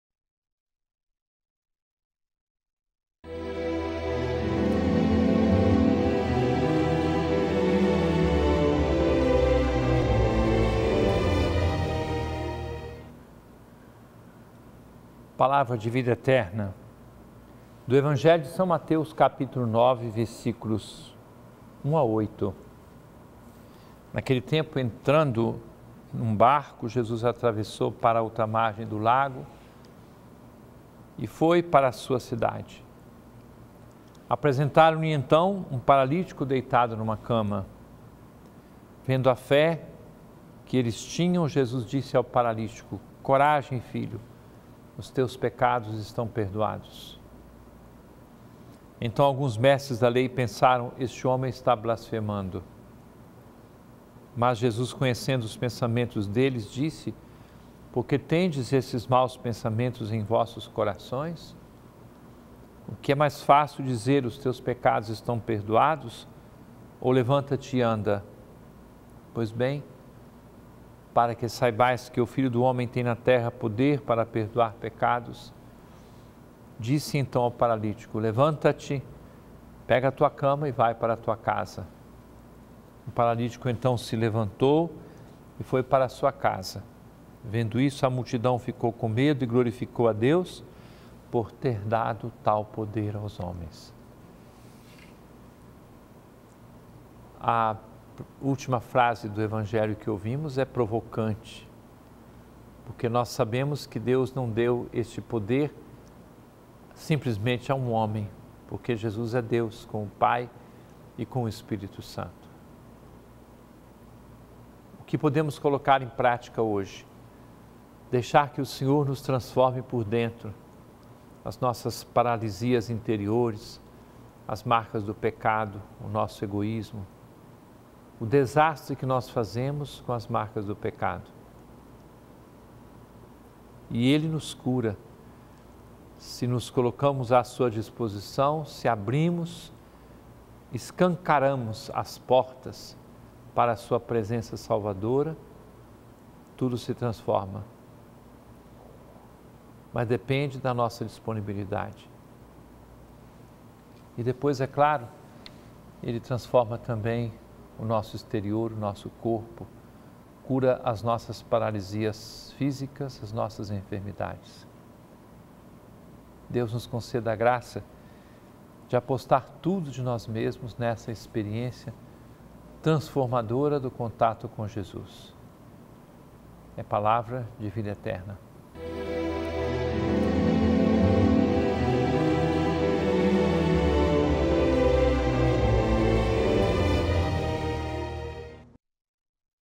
Palavra de Vida Eterna | São Mateus 9,1-8 05/07/2018
Naquele tempo, 1entrando em um barco, Jesus atravessou para a outra margem do lago e foi para a sua cidade. 2Apresentaram-lhe, então, um paralítico deitado numa cama. Vendo a fé que eles tinham, Jesus disse ao paralítico: “Coragem, filho, os teus pecados estão perdoados!”

3Então alguns mestres da Lei pensaram: “Esse homem está blasfemando!” 4Mas Jesus, conhecendo os pensamentos deles, disse: “Por que tendes esses maus pensamentos em vossos corações? 5O que é mais fácil, dizer: ‘Os teus pecados estão perdoados’, ou dizer: ‘Levanta-te e anda’?

6Pois bem, para que saibais que o Filho do Homem tem na terra poder para perdoar pecados, — disse, então, ao paralítico — “Levanta-te, pega a tua cama e vai para a tua casa”. 7O paralítico então se levantou, e foi para a sua casa. 8Vendo isso, a multidão ficou com medo e glorificou a Deus, por ter dado tal poder aos homens.